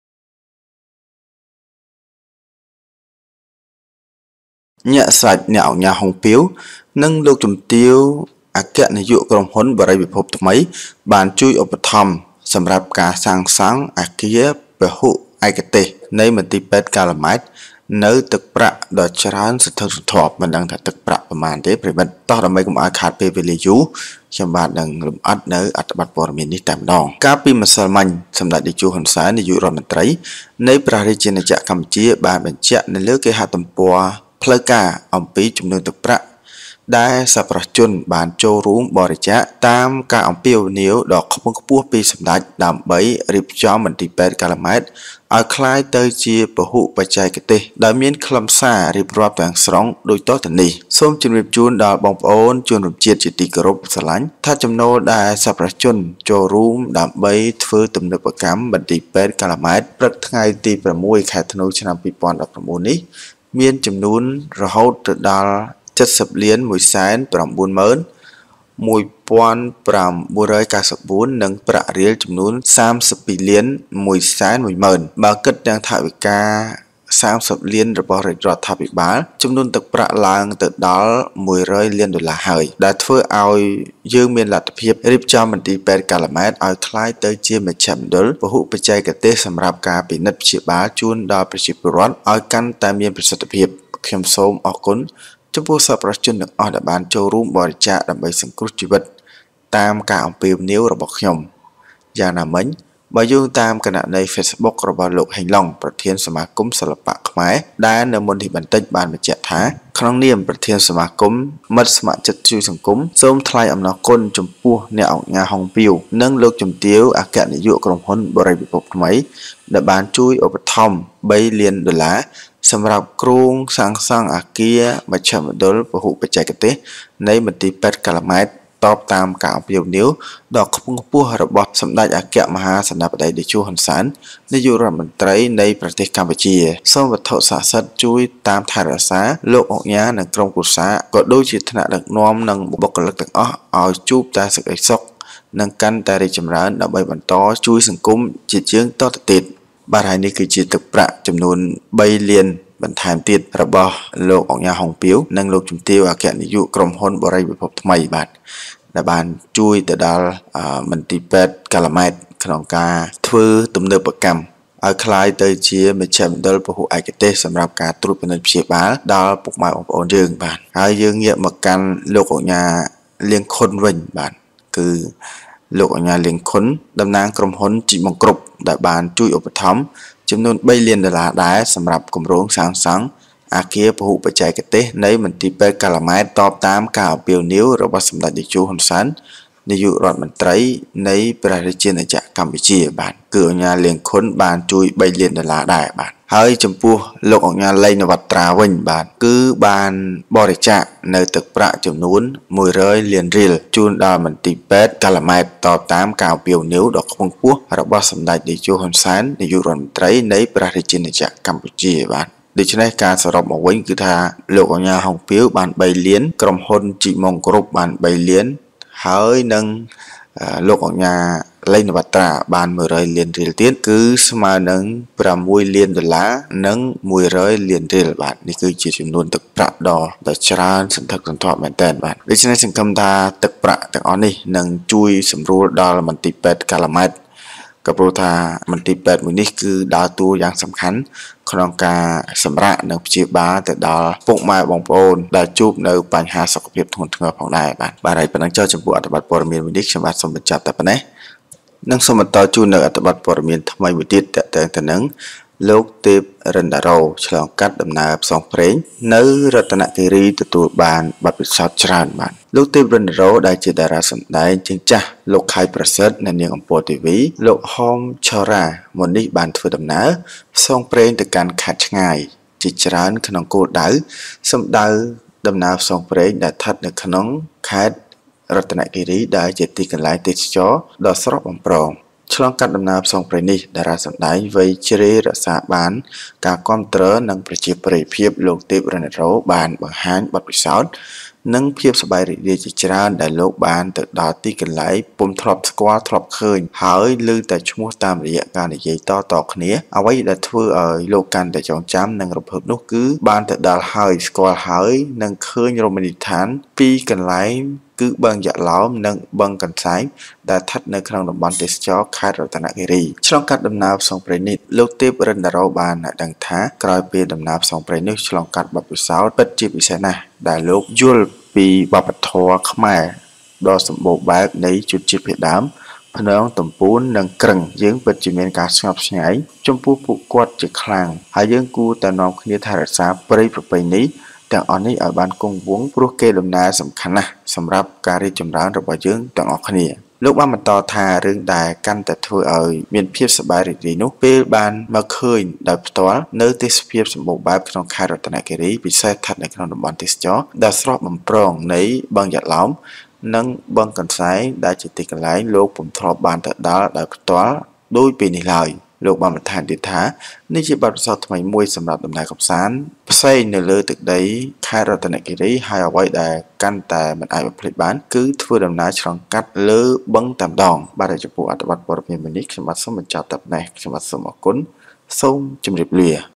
ញាក់សាច់!ឧកញ៉ា ហុង ពីវ,លាង ឃុន,ឡេង ណាវ៉ាត្រា បាននាំគ្នាជួយមន្ទីពេទ្យ កាល់ម៉ែត យ៉ាងច្រើនសន្ធឹកសន្ធាប់
អ្នកឧកញ៉ា ហុង ពីវ និងលោកជំទាវ អគ្គនាយកក្រុមហ៊ុន បុរីពិភពថ្មី​ និង
ក្រុមហ៊ុន ជីប ម៉ុង គ្រុប​ព្រមទាំង លោកឧកញ៉ា ឡេង ណាវ៉ាត្រា បានជួយ​ឧបត្ថម្ភ មន្ទីរពេទ្យកាល់ម៉ែតយ៉ាងច្រើនសន្ធឹកសន្ធាប់។